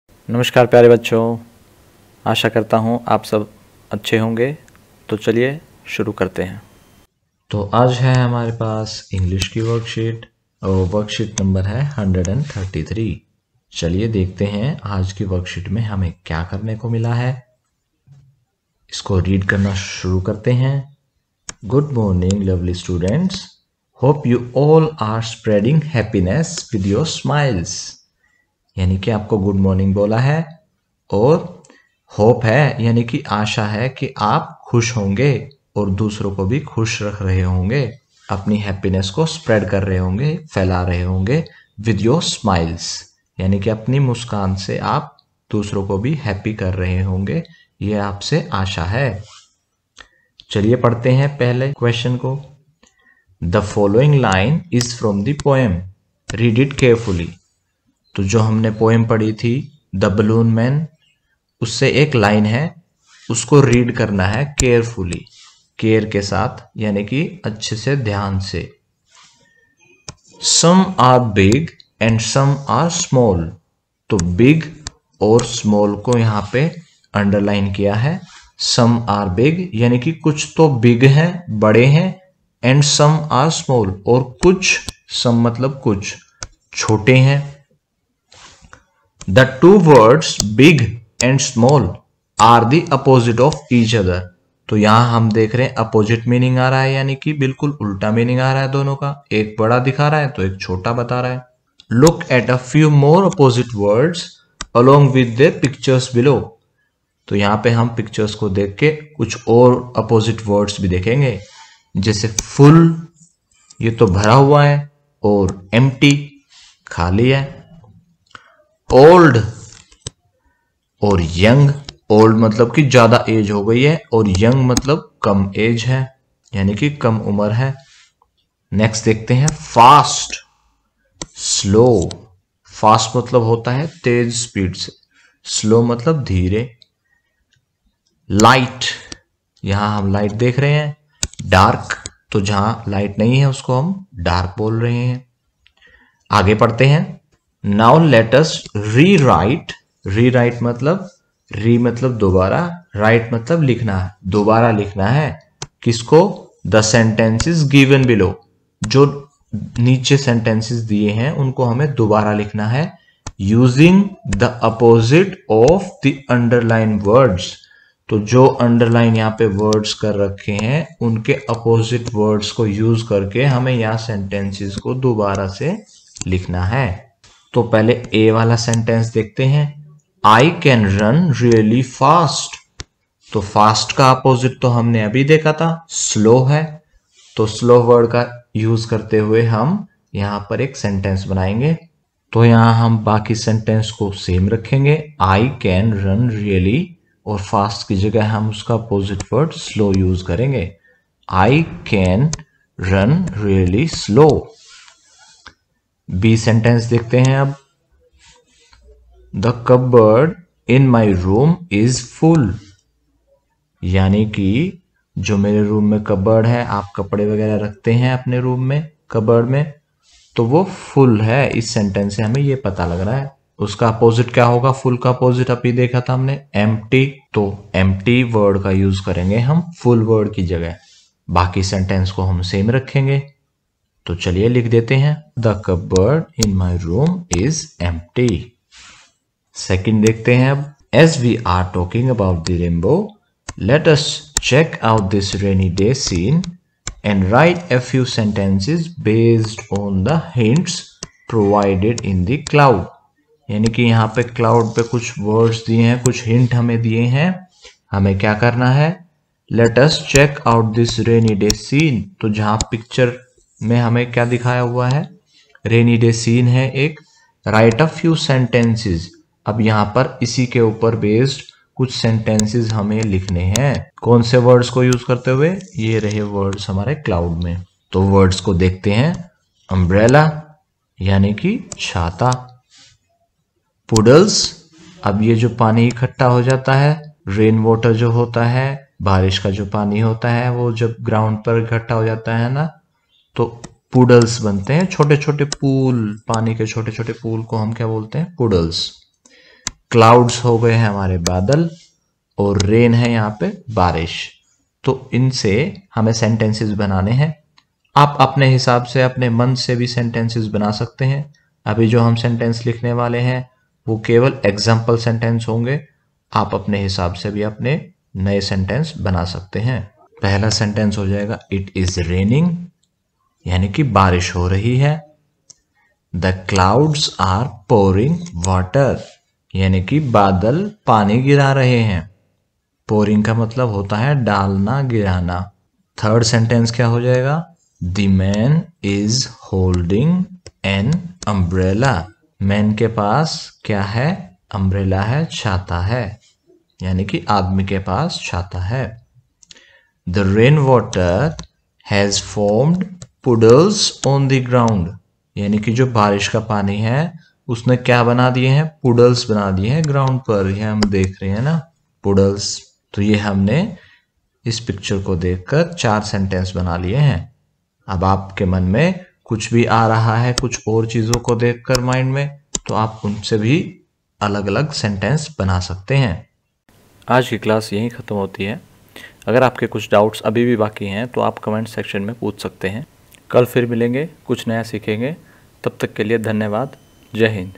Class 3 Worksheet 133 | Writing | The Balloon Man | English | 16 Dec 2020
Explanation for:  Class 3 Worksheet No. 133, Dated 16/12/2020, English Worksheet, Directorate of Education, GNCT of Delhi